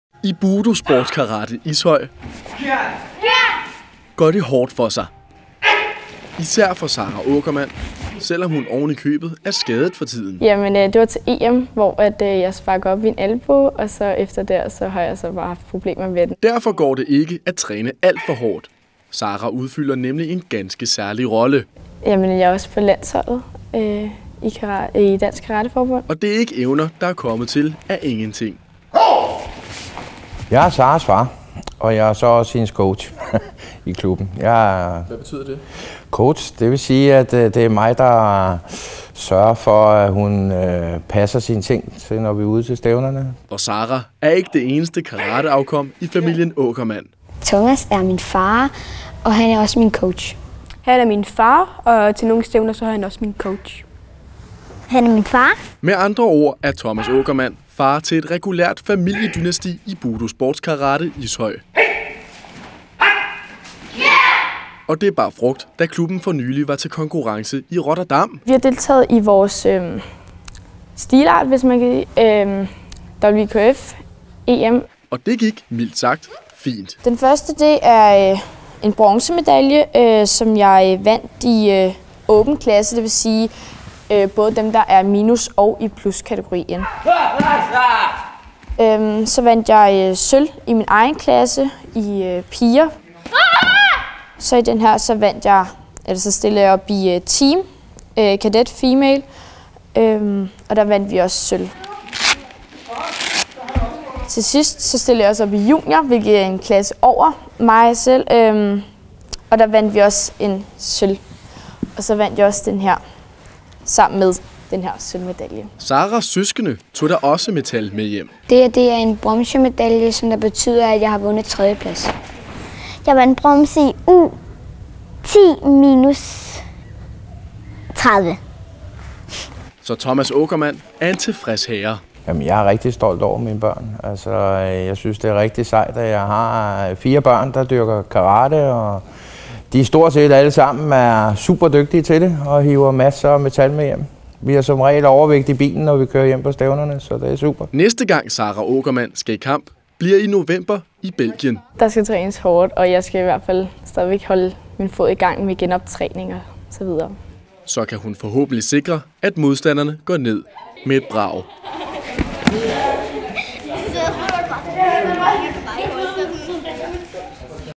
Lokal Karatehelt river medaljer hjem til Ishøj - TV-Ishøj
15-årige Sarah Aakerman er en stjerne på karatehimlen i Ishøj. Hun er på landsholdet og træner gerne trods skader. Og hun er ikke den eneste med efternavnet Aakerman, der lader sit liv forme efter den japanske kampkunst.
Se mere her.